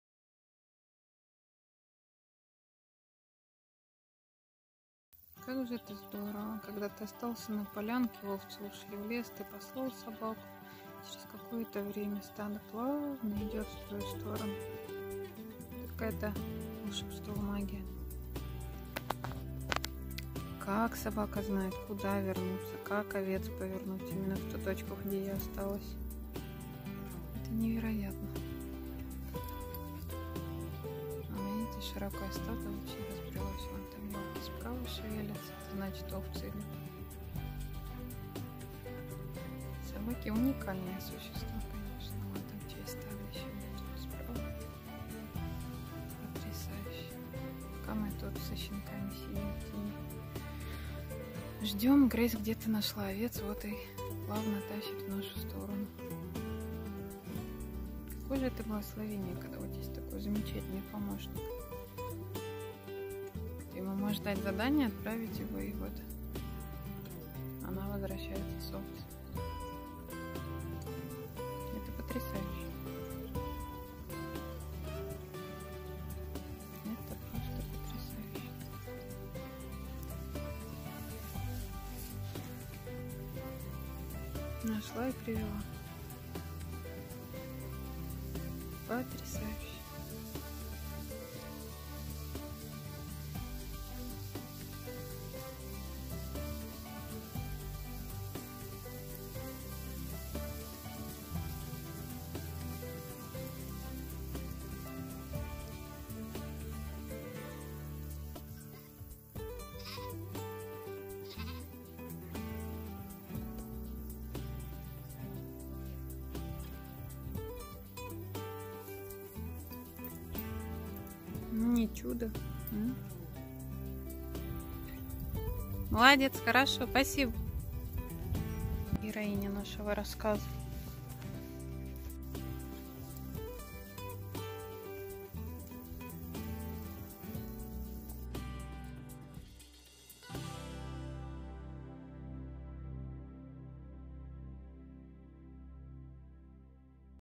Уникальная пастушья собака - поиск овец в лесу Kisses of Angel
Работа навыпасе стада овец в поле и среди деревьев
Питомник Бордер Колли
Kisses of Angel

Дрессировочный Центр Династия
 Украина Киев
 Тренера Артём Подлин и Немченко Марина